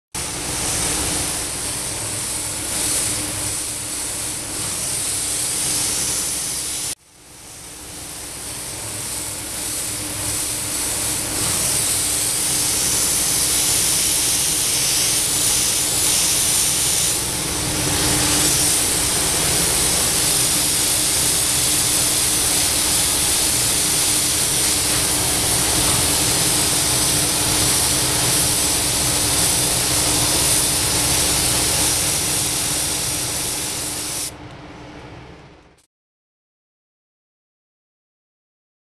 Perlweiss-Spezial-Effekte lackieren
Die Restauration von DON CAMARO mit Perlweiss Spezial Effekt